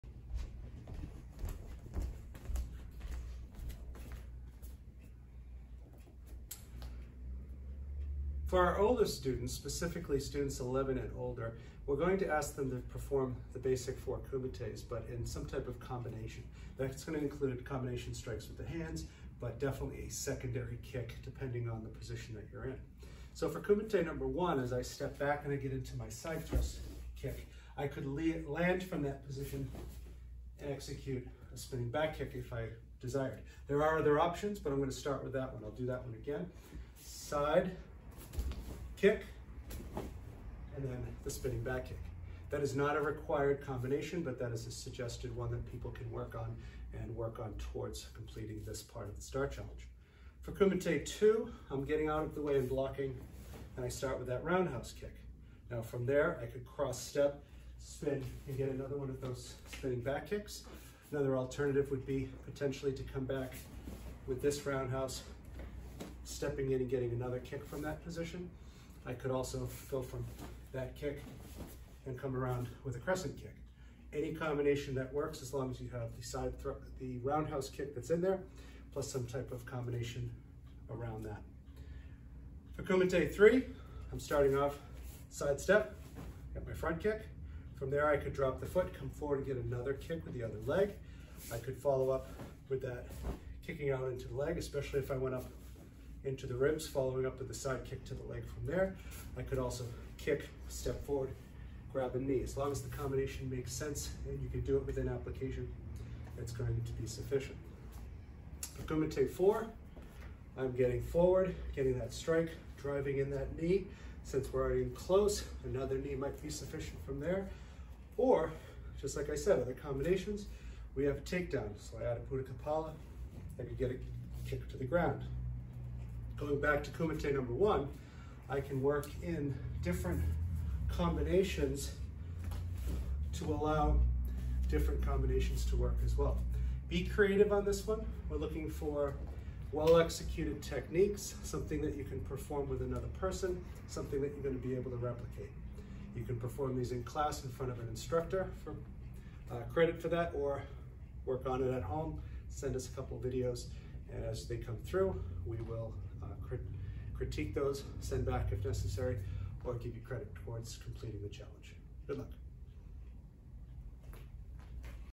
STAR Challenge #31 Ages 11+
This Red STAR (Martial Arts) Challenge continues to focus on kicking.  Students, ages 11+, are expected to demonstrate kumite (self defense) 1-4 with the appropriate kicks and combinations.  In addition to the basic kumite kicks (side kick, roundhouse kick, front snap kick, and knee), students must add more techniques and follow-ups and demonstrate these against an opponent.

Students must show us in class or send a video demonstration of the techniques.